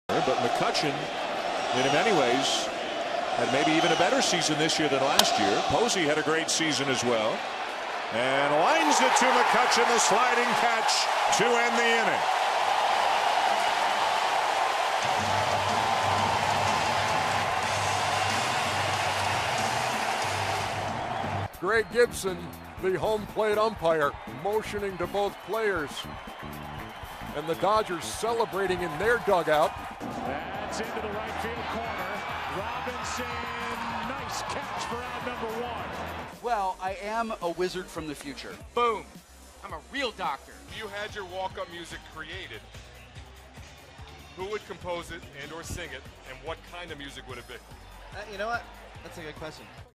Cutch slides in center for slick grab
10/1/14: Andrew McCutchen makes a slick sliding grab in center field to take a hit away from Buster Posey